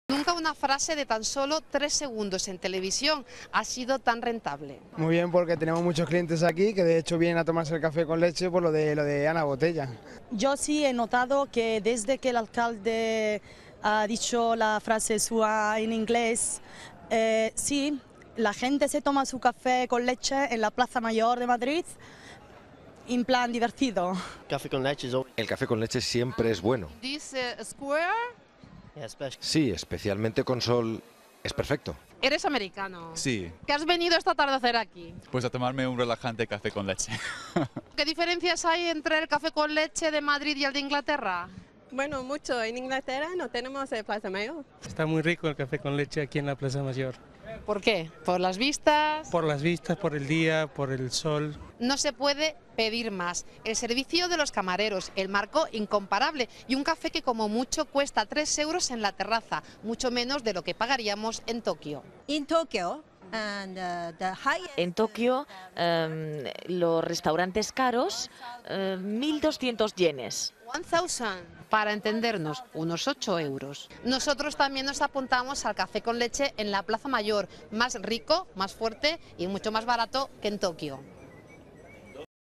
A relaxing cup of café con leche in Plaza Mayor
Es una de las frase más famosas del momento. La utilizó la alcaldesa de Madrid, Ana Botella, en la última presentación de la capital durante la sesión del COI en la que se eligió la ciudad organizadora de los Juegos de 2020. Finalmente Madrid no consiguió las ansiadas Olimpiadas, pero parece que las curiosas palabras han servido para animar el consumo de más "relaxing cups of" café con leche in Plaza Mayor.